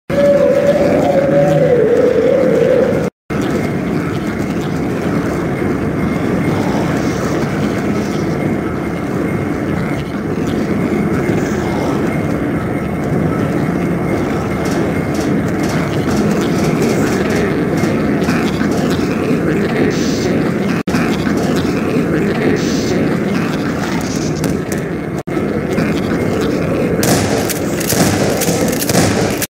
Moron
A very stupid Zombie...also a teaser for Monday's episode.